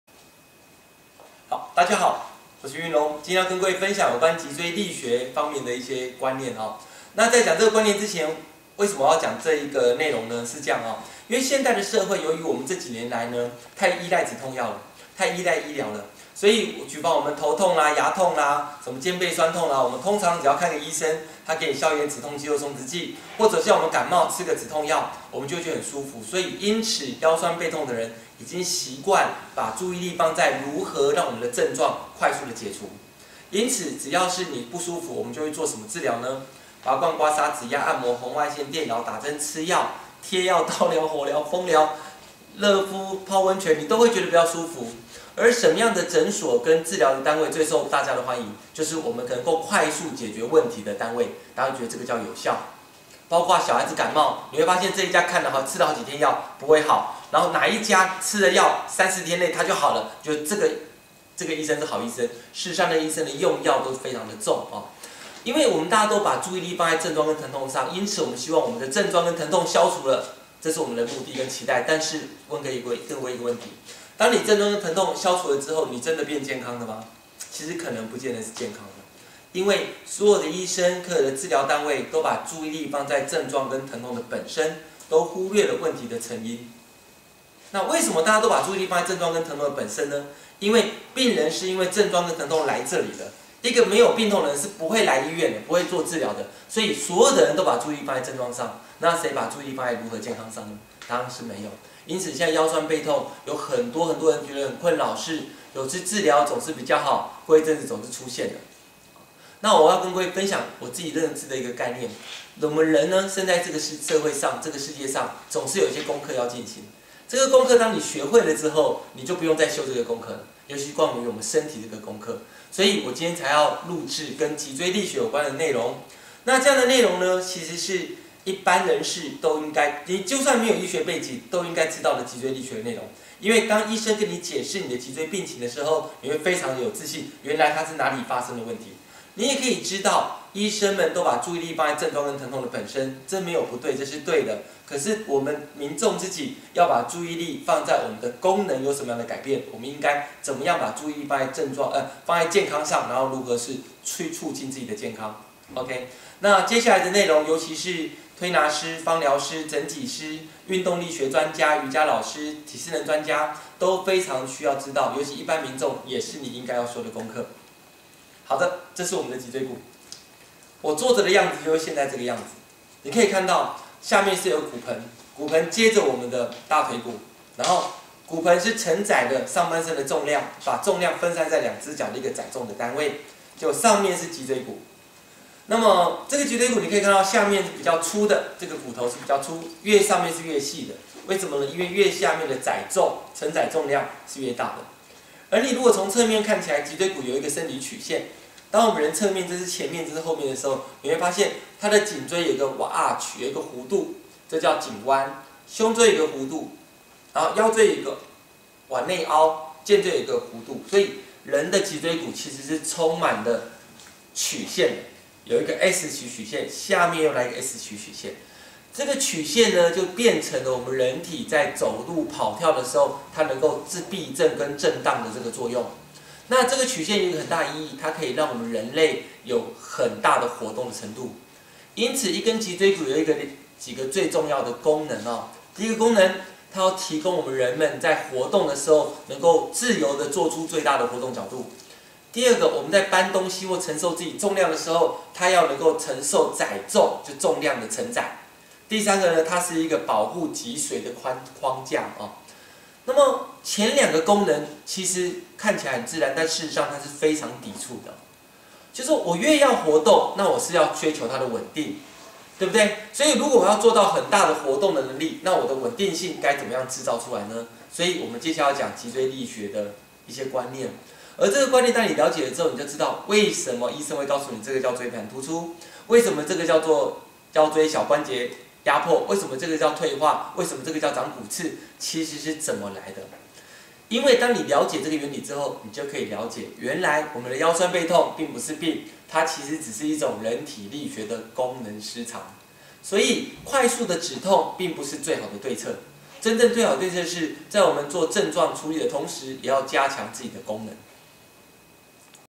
為何每個人都該認識基本的脊椎力學原理？ 脊椎力學系列01
「疾病管理靠醫療，健康促進靠自己」
醫生負責的是治療疾病，但關於脊椎的健康，其實更需要每個人去了解基本的觀念，這樣你才能了解快速止痛並非最佳的選擇，而是同時加強自己的身體功能。

更多脊椎保健教學課程、輔助用品歡迎至身體智慧官網報名與訂購：